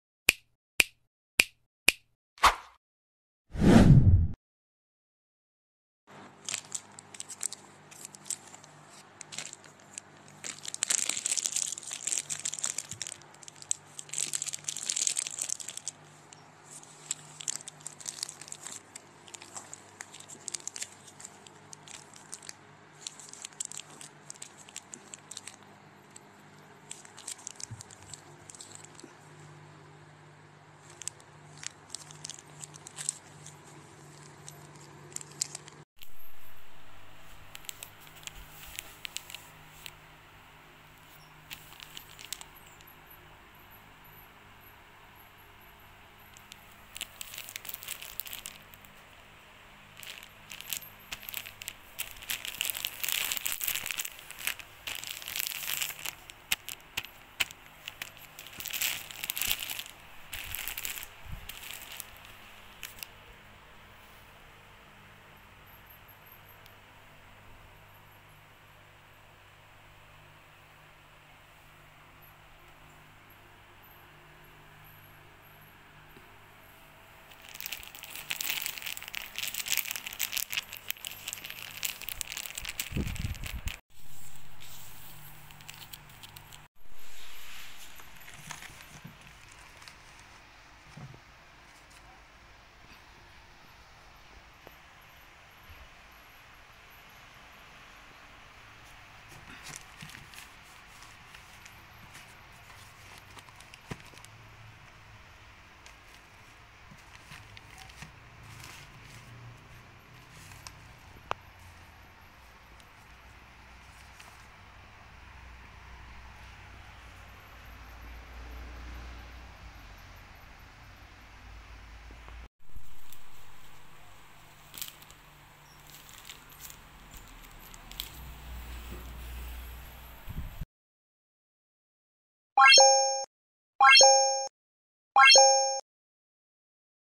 unboxing my facial tissue
Hai Assalamualaikum Semua ❤️
Today I have shared my unboxing for today 😊

so cute tissues and it was limited edition
just to make my life easy and portable 😊

Hopefully you enjoyed my Video's 😉
Dropped your comments 👇

what is your favourite marvel?